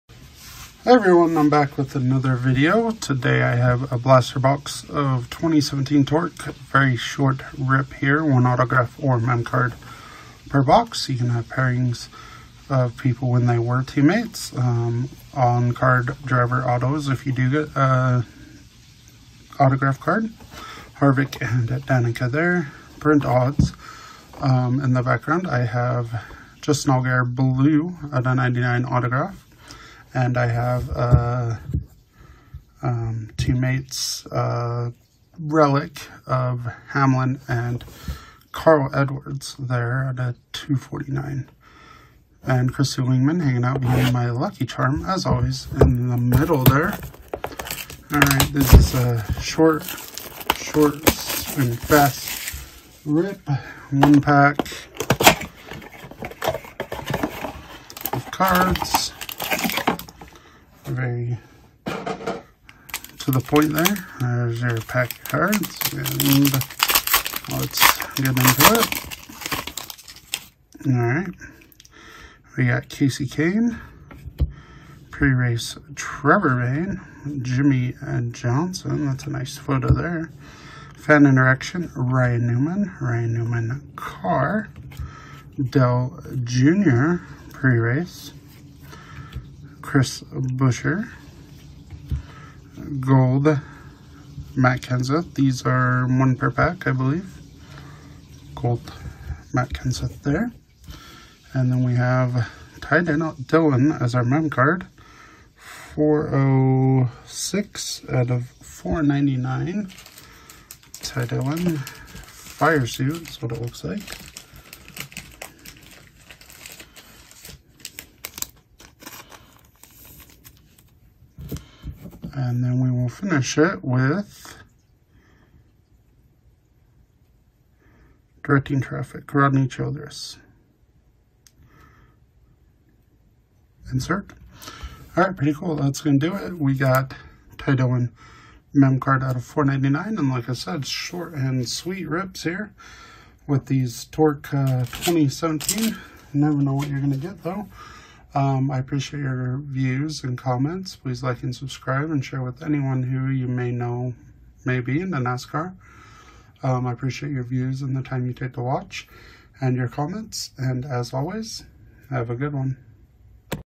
2017 Torque Blaster Box
I open a 2017 Torque Blaster Box.